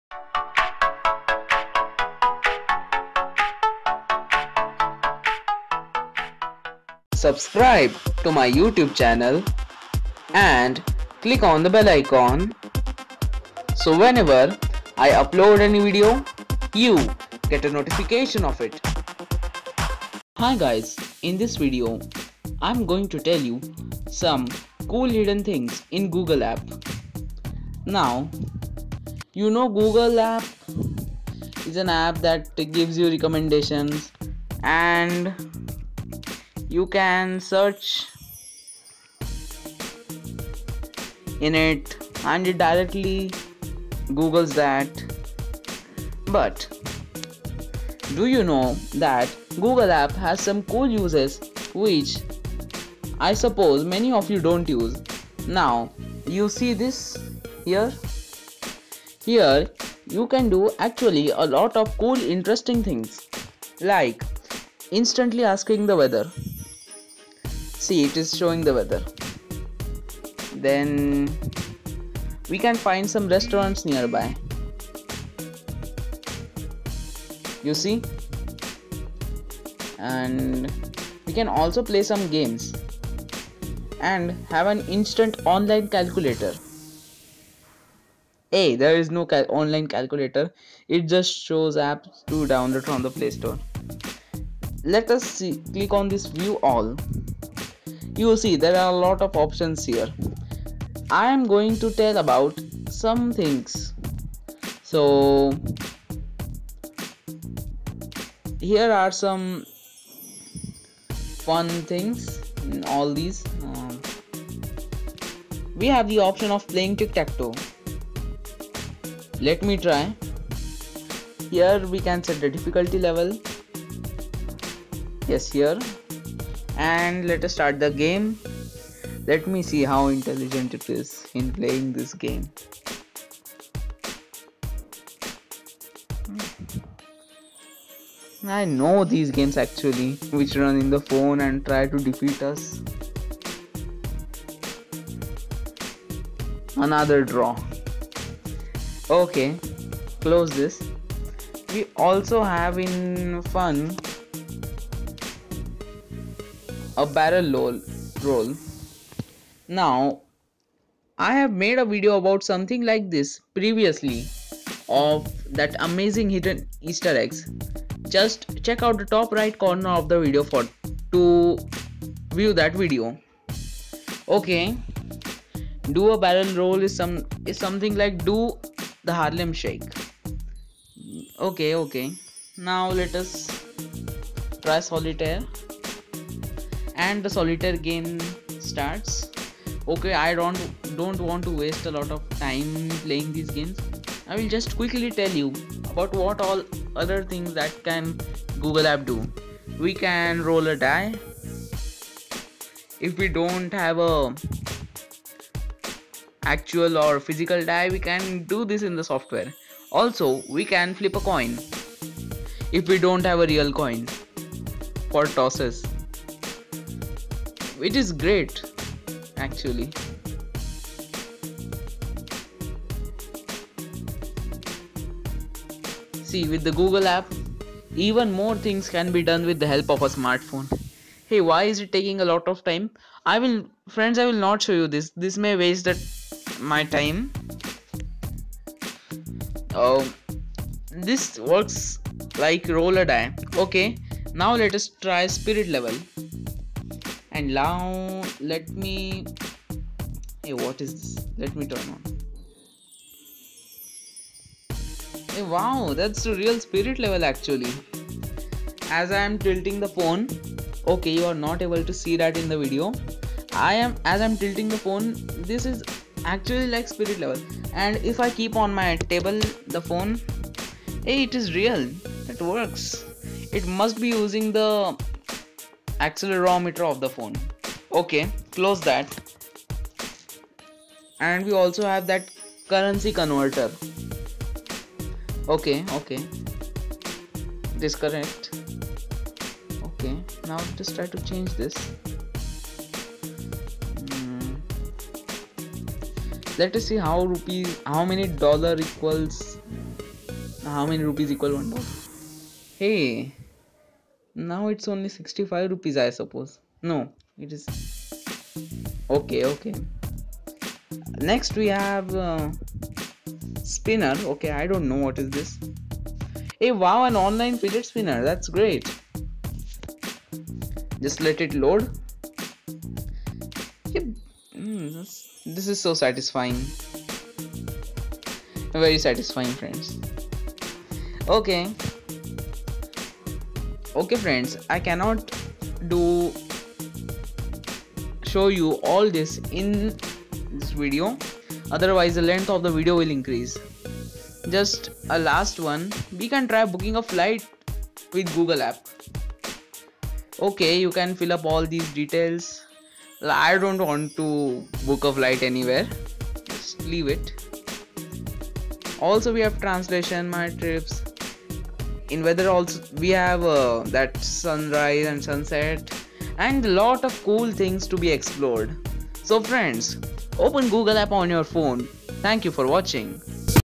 Cool hidden things in Google App
In this video I told you some cool and hidden things in the google app which can be very useful at times.